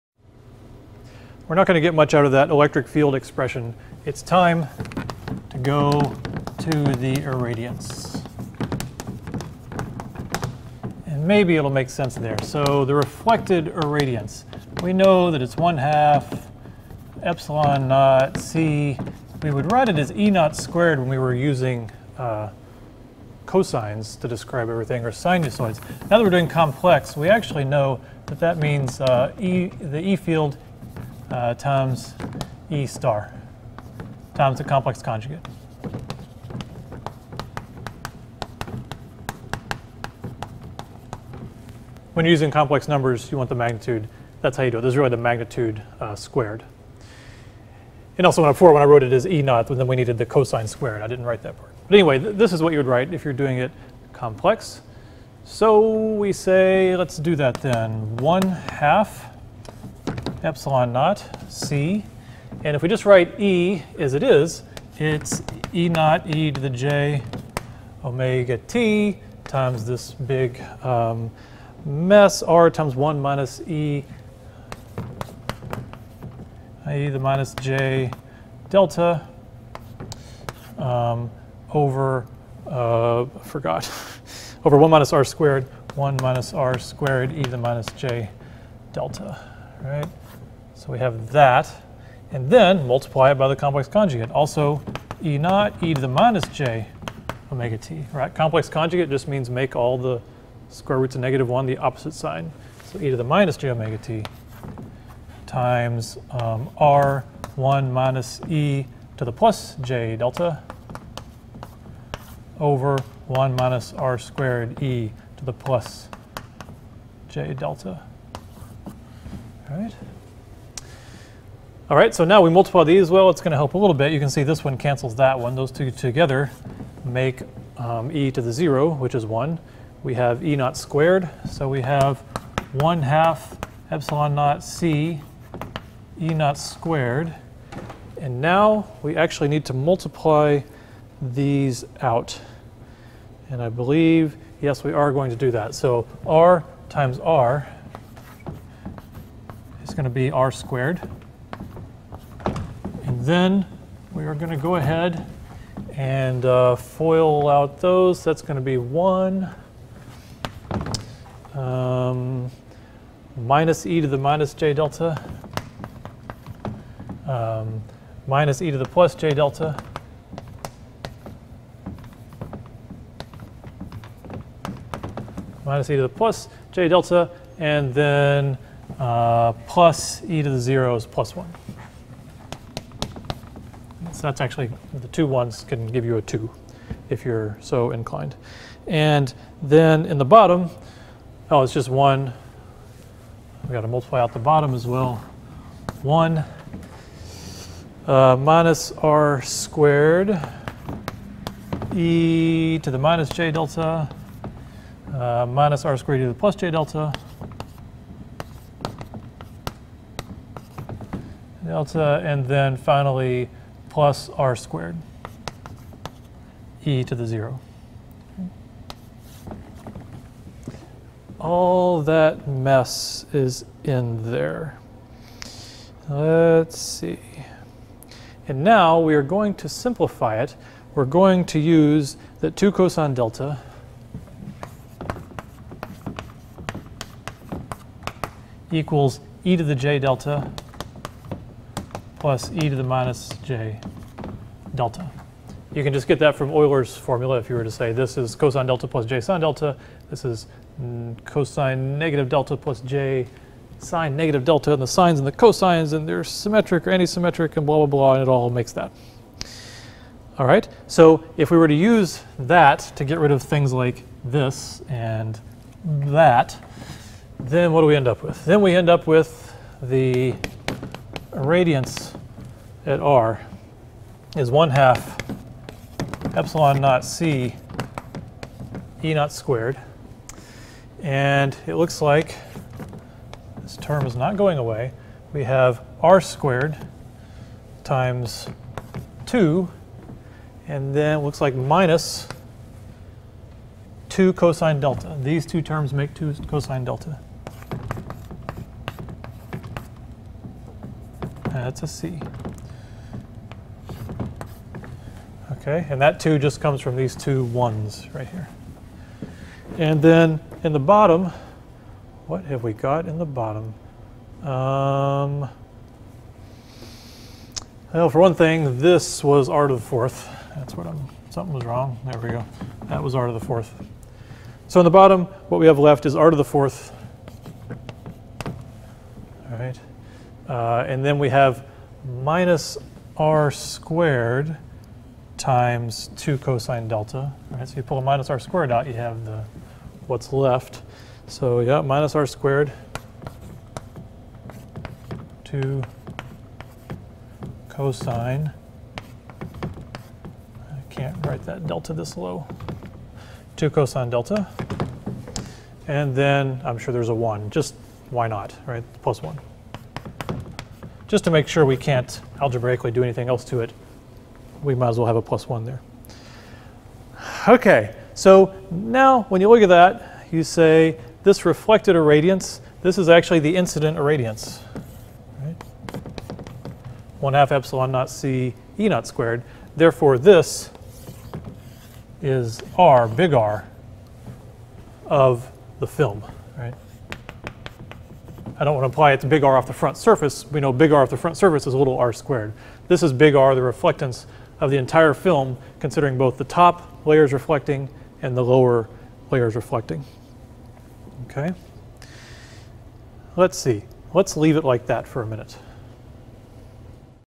PHYS 201 | Multiple Beam Interference 5
Of course, we observe irradiance, so here we square the complex field to calculate it.